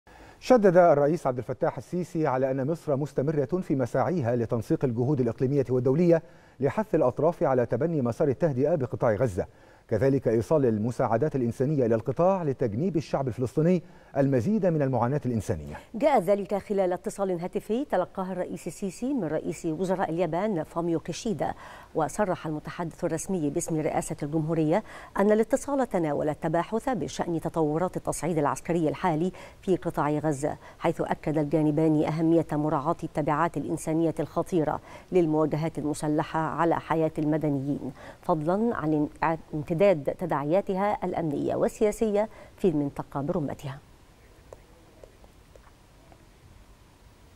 الرئيس السيسي يشدد على استمرار المساعي المصرية لتنسيق الجهود الإقليمية لتبني مسار التهدئة بقطاع غزة
الرئيس السيسي يشدد على استمرار المساعي المصرية لتنسيق الجهود الإقليمية والدولية لتبني مسار التهدئة بقطاع غزة